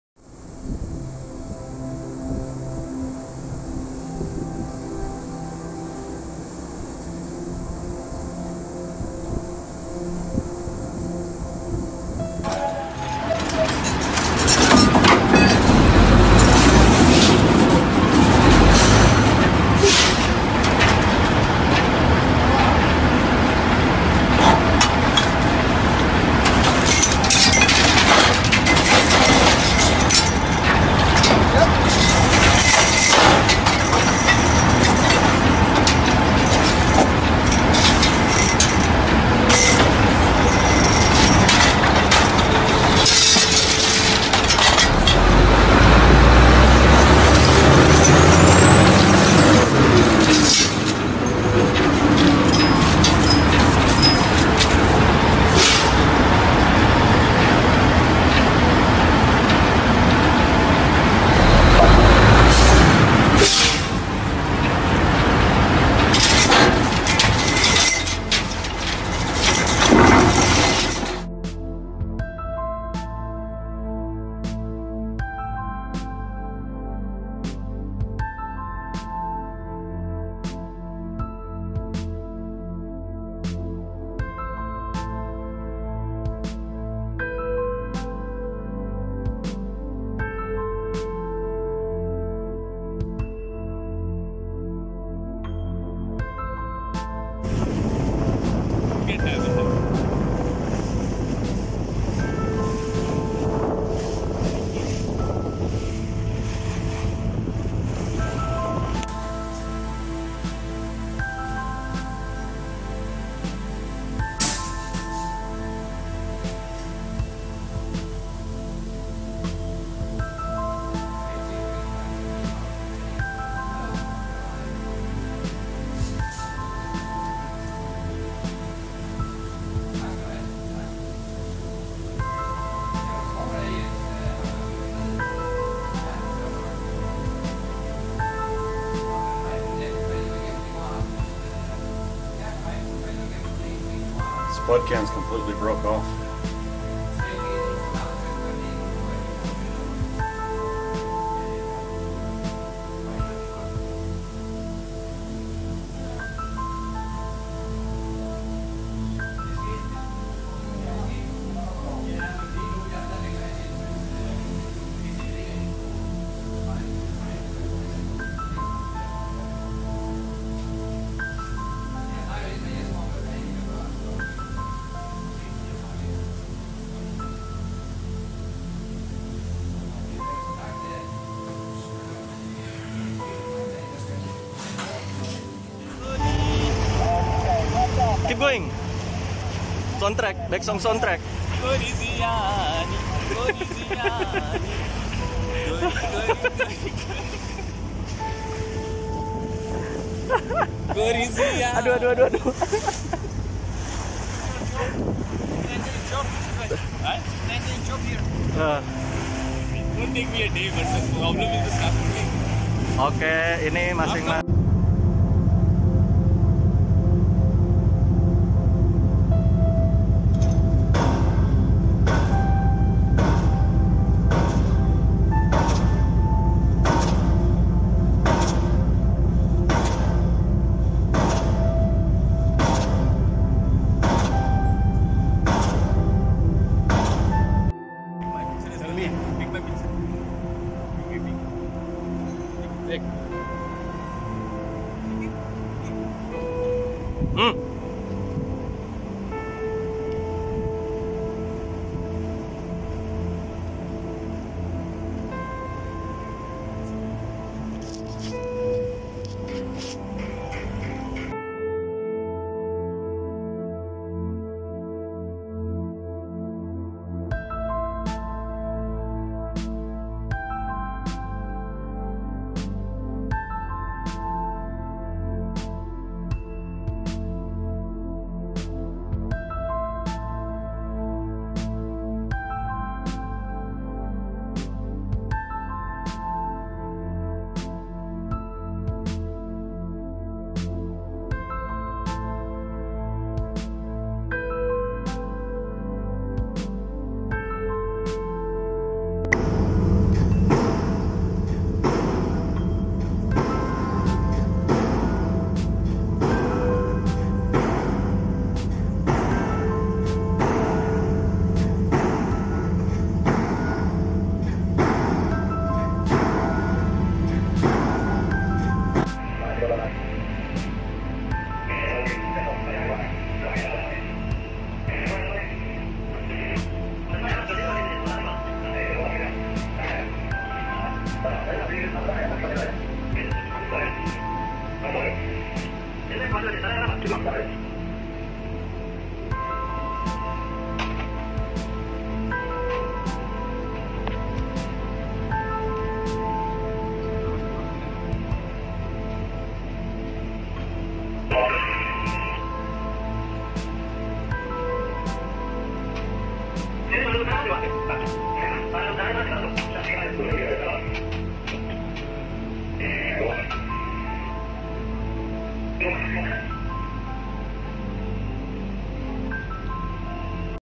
Life on an Offshore Oil Rig
Living and working conditions on an offshore oil rig could be tough.
Although the majority of oil and gas industry jobs are located onshore, careers in the offshore industry are extremely varied.

The Finest Hours Ch.

The video content has been made available for informational & educational purposes only